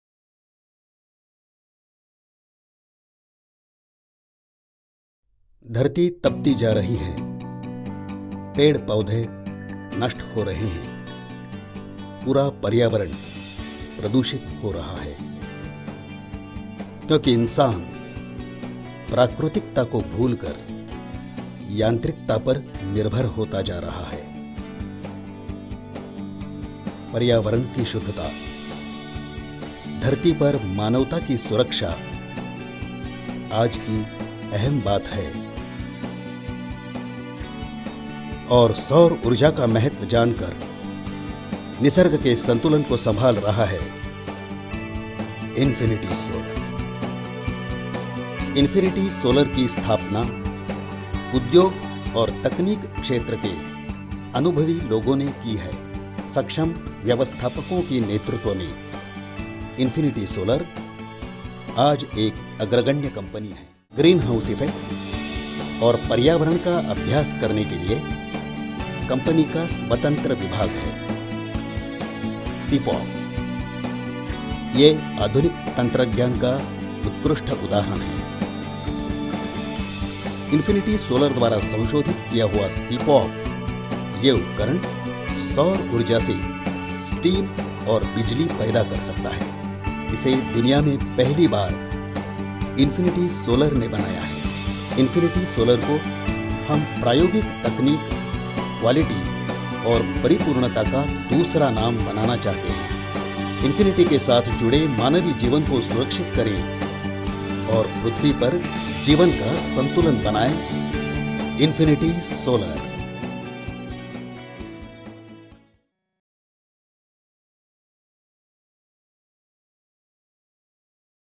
Infiniti Solar SEPOGS
A new concept design and created by Solar EnergieTechnik Ltd. which can generate power 24x7 with the help of Solar and Therma a Unique Concept which can change the World and can reduce the power outage issue and bring in more revenew and also generate employement  - Infiniti Solar Energy Power Generation System (SEPOGS)